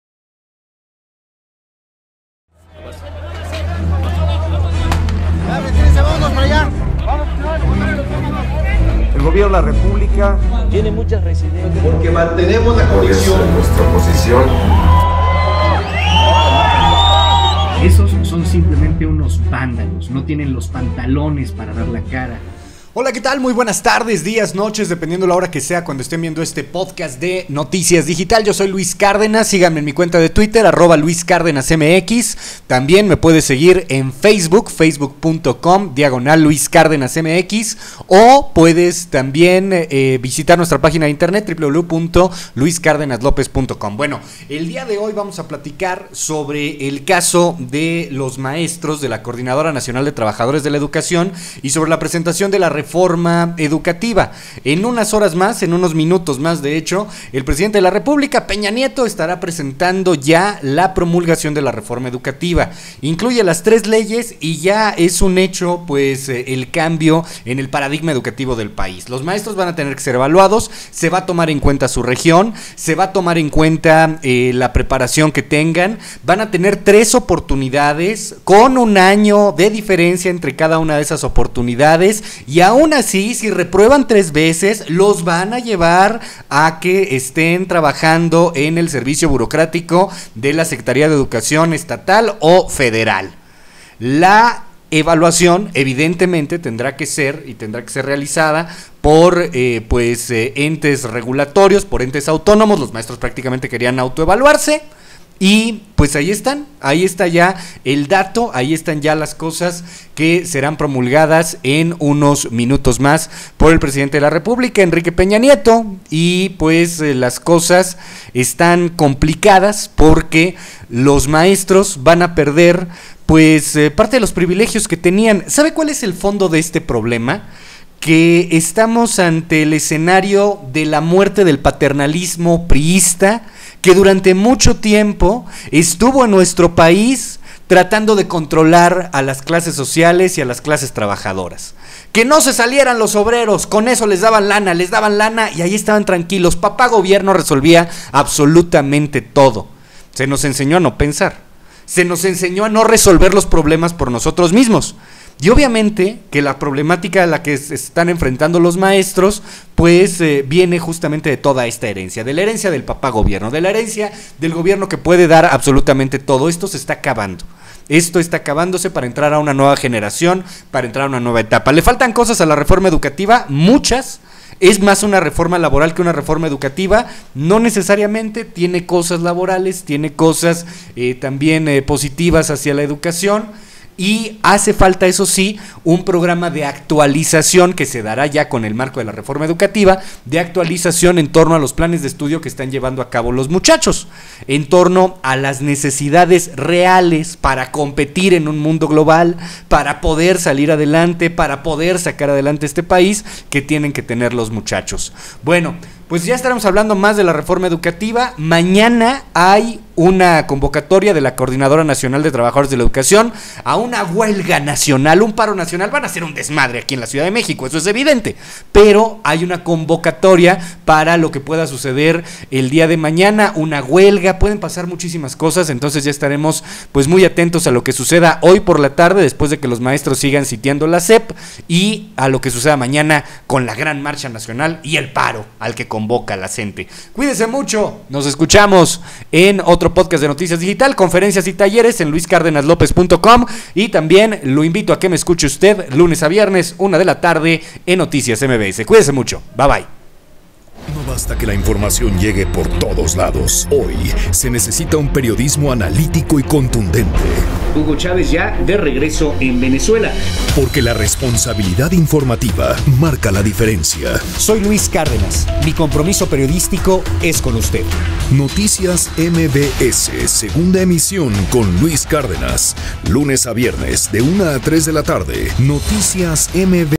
CNTE y la Reforma Educativa
Integrantes del Coordinadora Nacional de Trabajadores de la Educación continúan con las protestas en contra de las leyes secundarias de la reforma educativa y en esta ocasión, los docentes se manifestaron fuera de la Secretaría de Educación Pública.

Mediante su cuenta de Twitter, la Secretaría de Seguridad Pública del Distrito Federal (SSPDF) informó que los maestros se encuentran en las inmediaciones de la SEP por lo que pidió tomar precauciones.